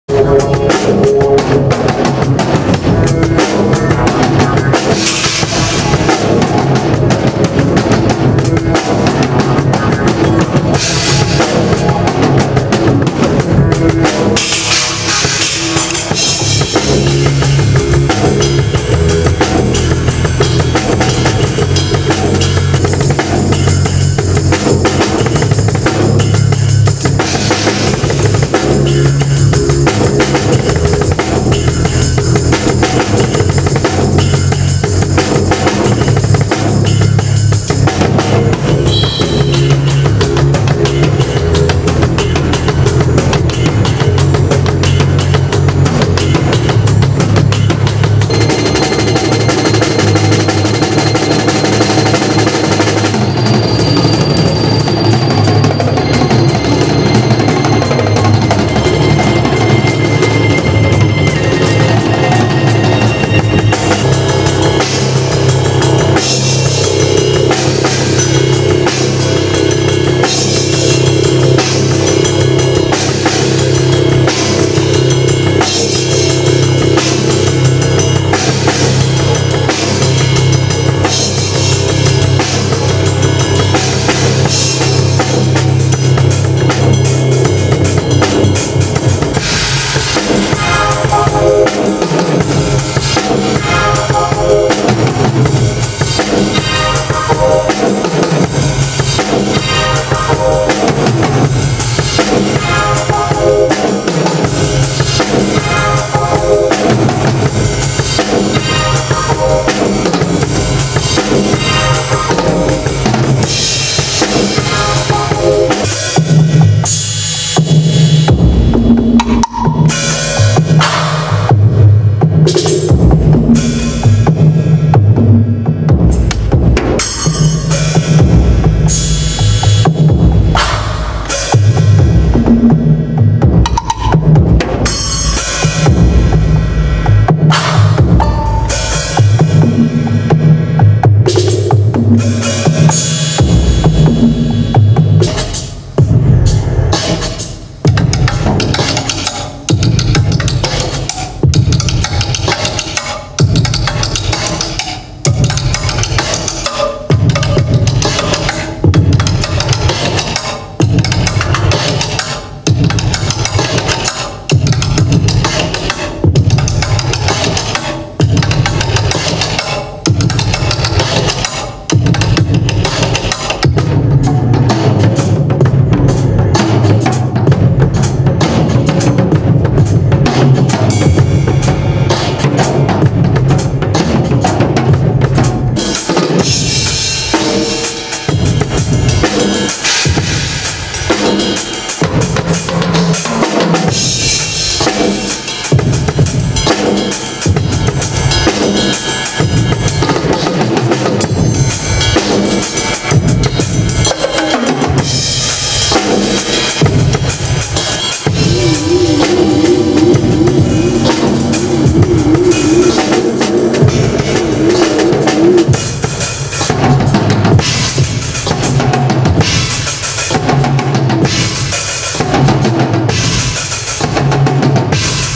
Fiske (demo)
3 ish minute highlight clips of performance @ Fiske Planetarium